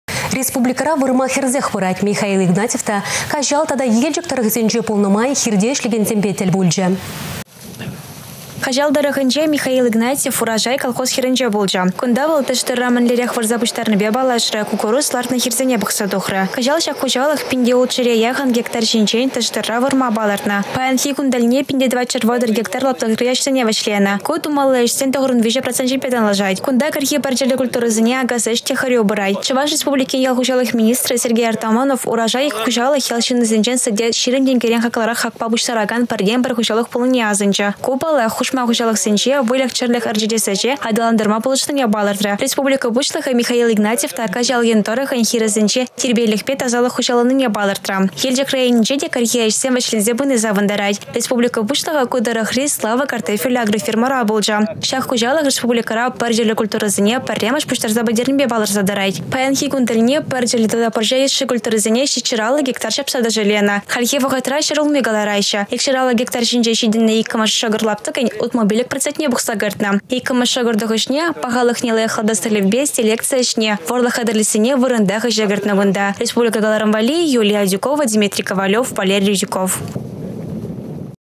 Михаил Игнатьев Каçал тата Елчĕк тăрĕхĕсенчи кĕрхи ĕçсемпе паллашрĕ
Республикăра вырма хĕрсех пырать. Михаил Игнатьев та Каçал тата Елчĕк тăрăхĕсенче пулнă май хирте ĕçлекенсемпе тĕл пулчĕ.